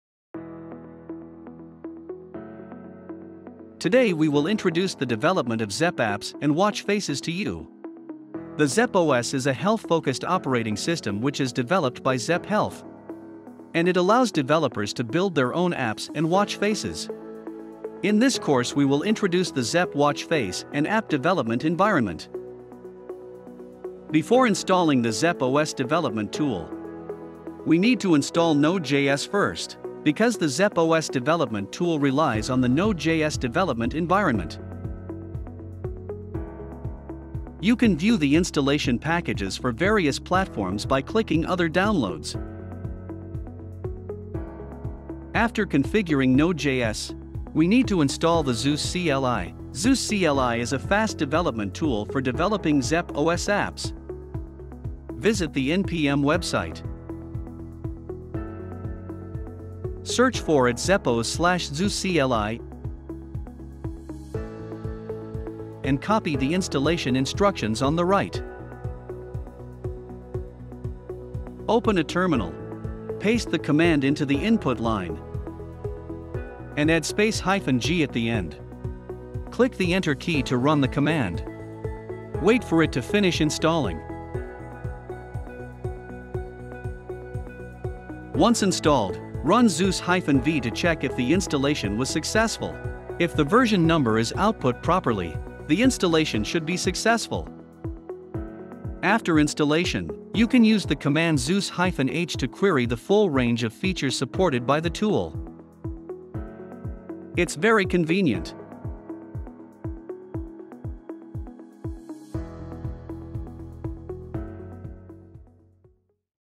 Tutorial | Zepp OS Zeus CLI Installation Guide | Amazfit
Find out how to install the Zepp OS development tool, Zeus CLI, to begin developing your own watch faces and apps that are compatible with Amazfit watches powered by the ZeppOS.